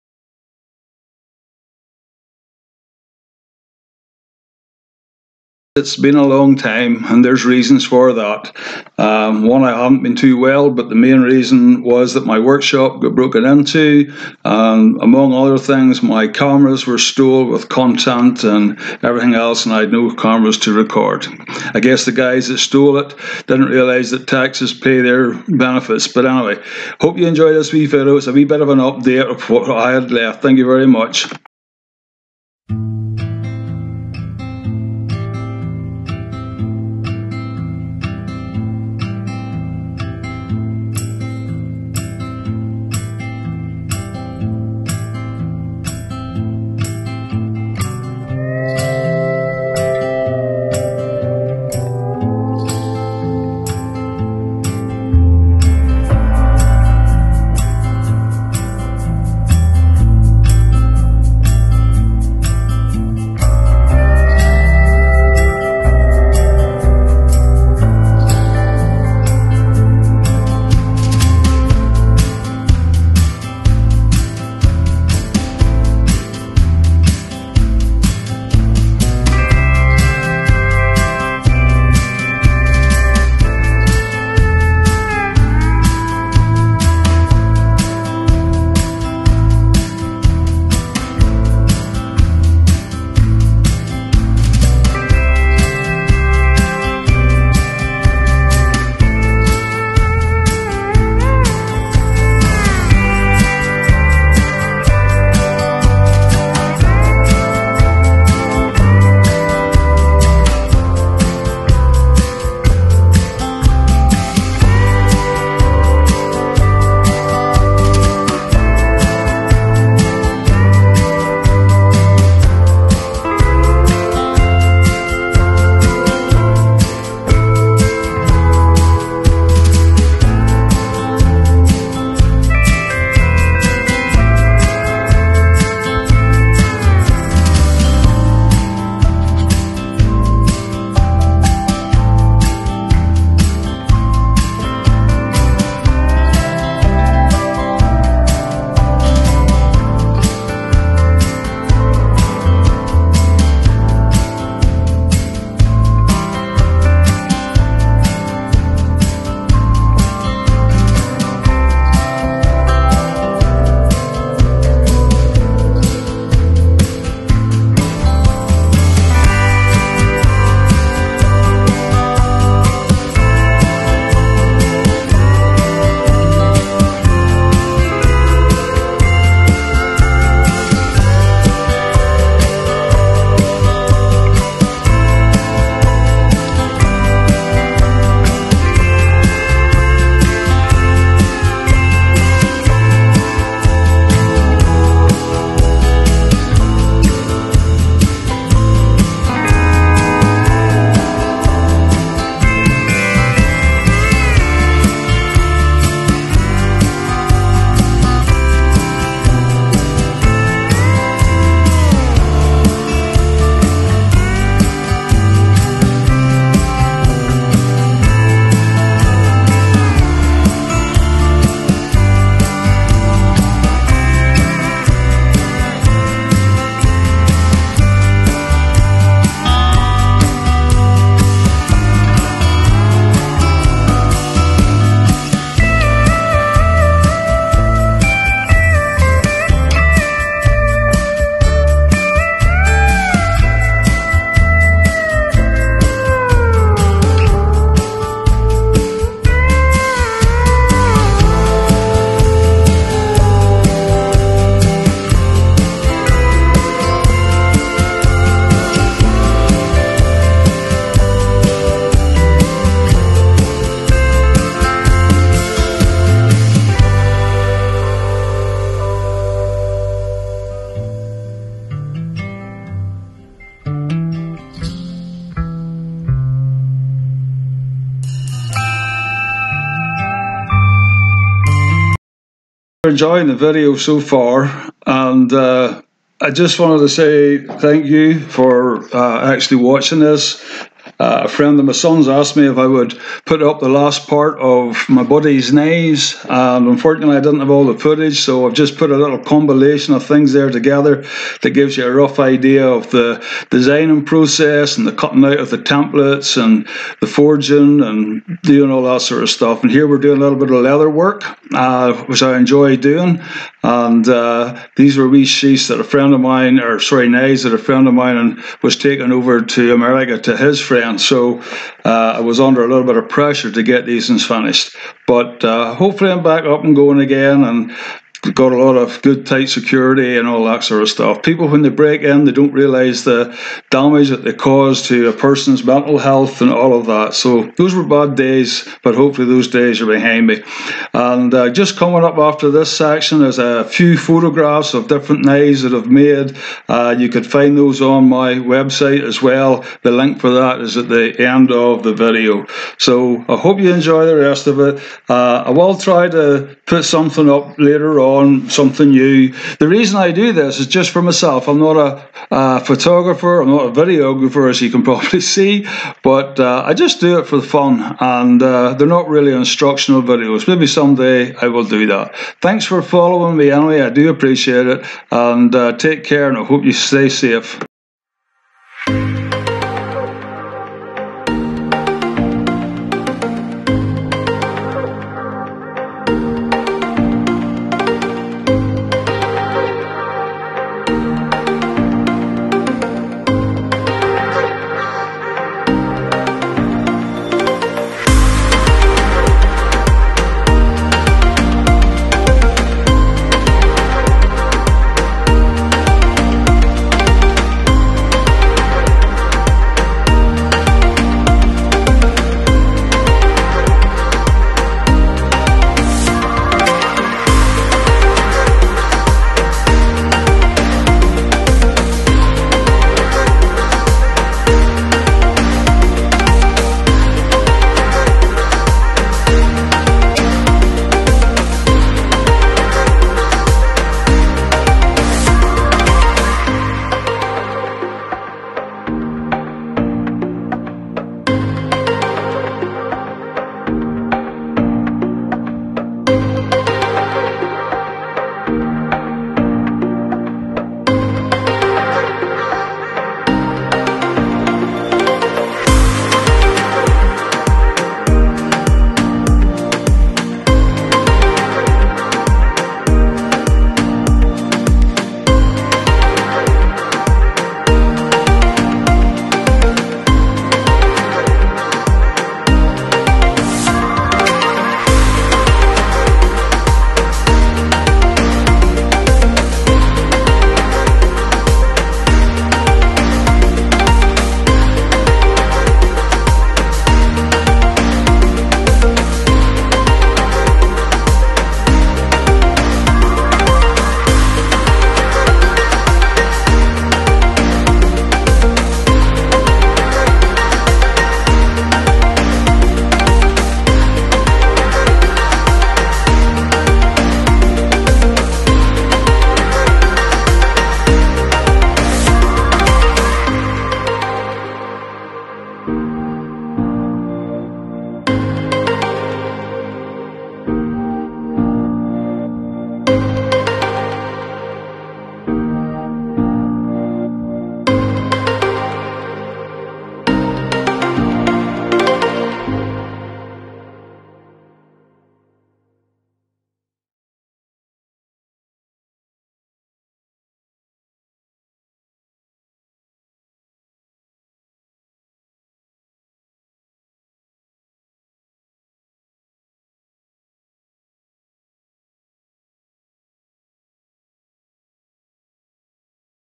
Buddies knives Pt 5 sort of plus update
This video was meant to be Pt 5 of a mini series.  However, most of the content was on the cameras that were stolen.  So I have put together a video  of relevant parts to make a video and listed all my photos to this also.  While collating content, I realised a friend who died with cancer helped take some of the photos.  So I dedicated this video to him.  You will see him right at the end of the video, please hang in there to meet David.